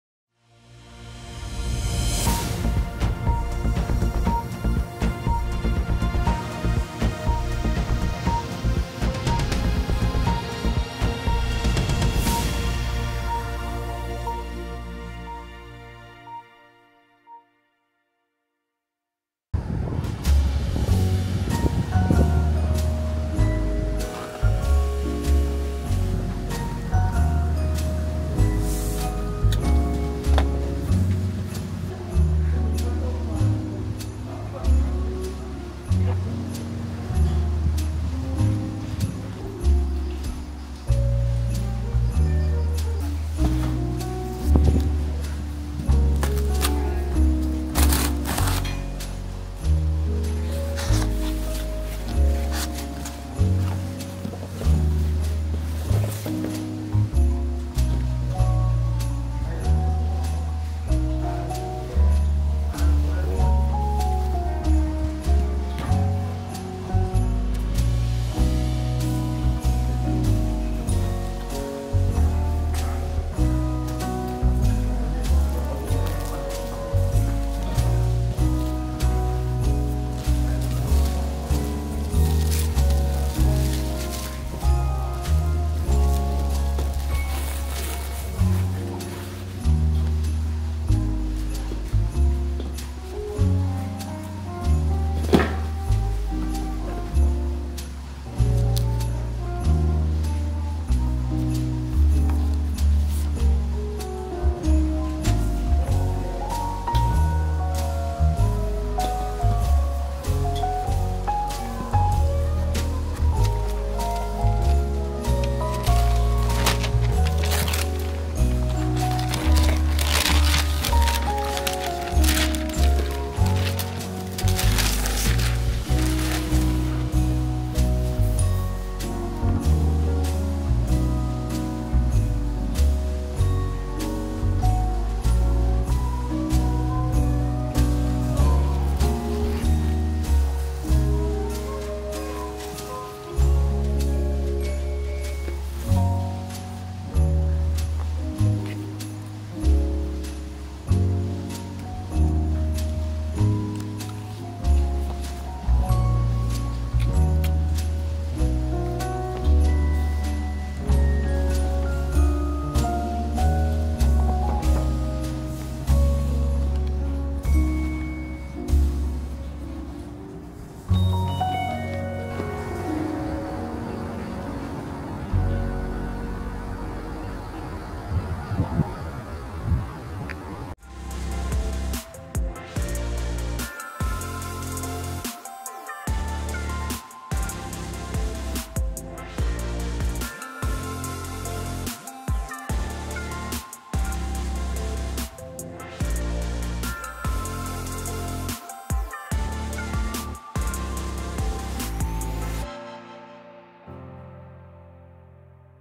비오는 노들섬 맥북 언박싱!
어제까지만 해도 36도였는데,
갑자기 이렇게 추워지는군요

갑작스런 선선한 날씨를 즐기려 노들섬에
맥북과 함께 나왔습니다.

바로찍고 바로 올려봅니다.

이렇게 새맥북과 함께 출연해 주신 홍시 작가님께 감사드립니다.